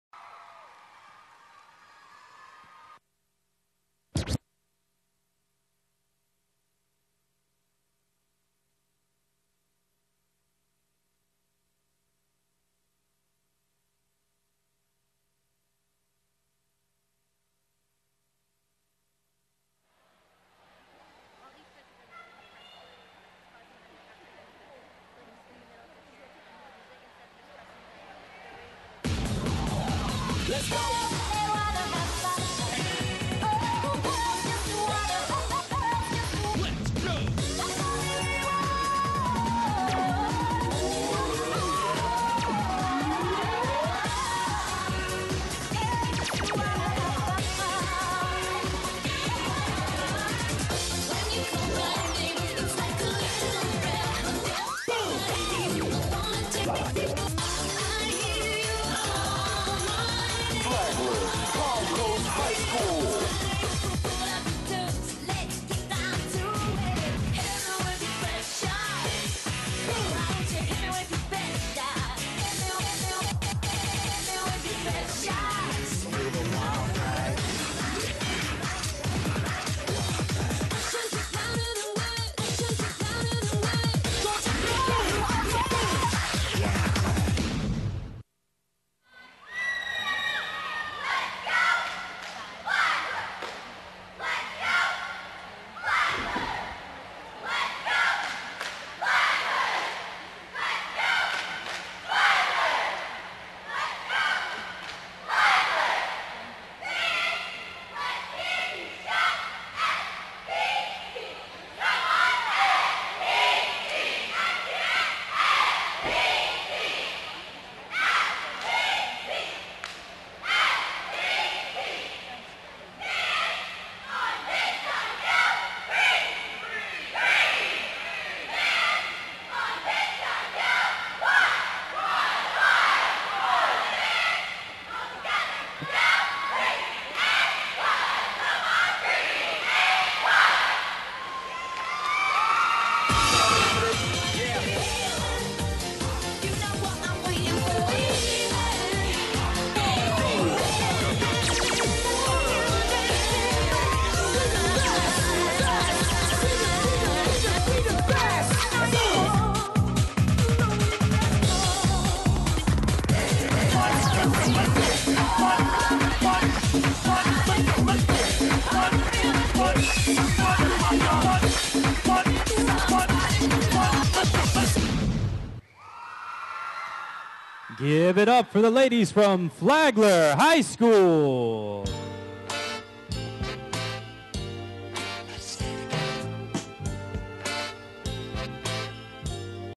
Flagler High's Small Non-Tumbling Team Preliminary Performance
Flagler High School's routine from the FHSAA Division 2A Small Non-Tumbling Competitive Cheer Championship Finals from Silver Spurs Arena in Kissimmee, Florida.